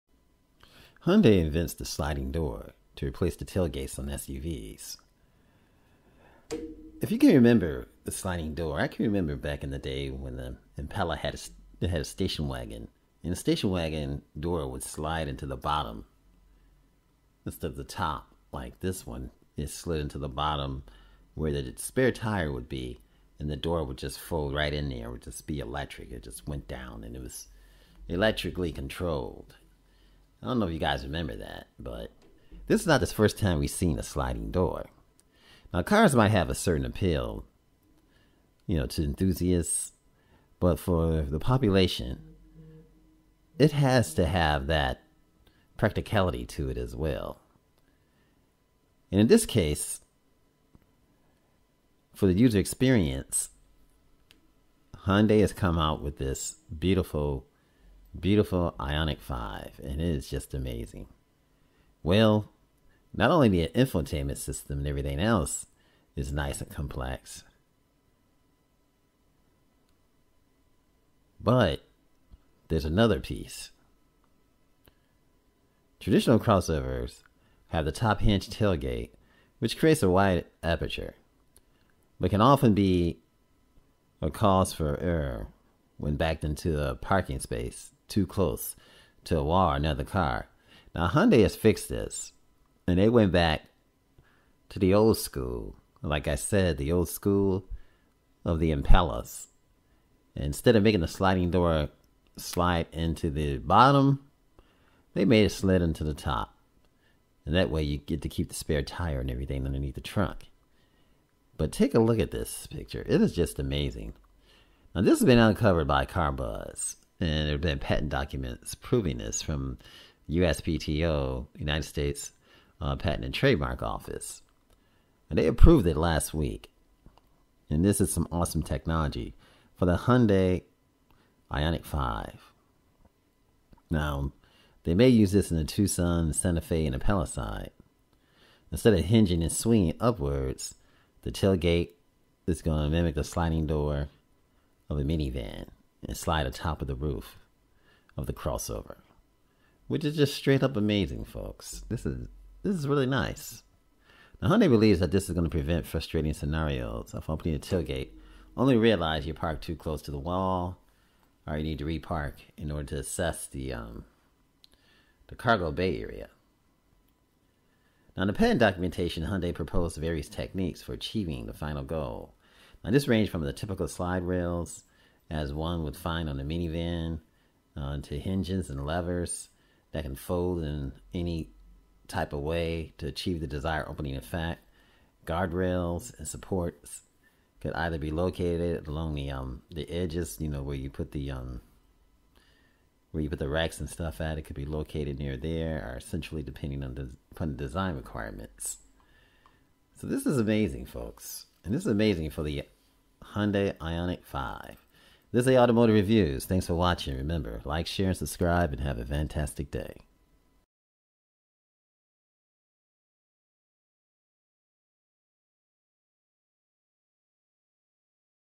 Hyundai Invents Sliding Door To Replace Tailgates On SUVs
But sliding tailgates are the future of practicality.

Cars might have a certain appeal as thrill machines for enthusiasts, but for the bulk of the population that uses them, they're first and foremost appliances. That's important, as the user experience can be severely improved, or hampered, by an appliance that is inconvenient to use. It's why complex infotainment systems have caused poor initial quality ratings among new car buyers and why we've raved about machines like the Polestar 2 that make life so easy by removing additional complexity. Hyundai has now come up with what it believes is the next step in making cars - particularly crossovers - easier to live with.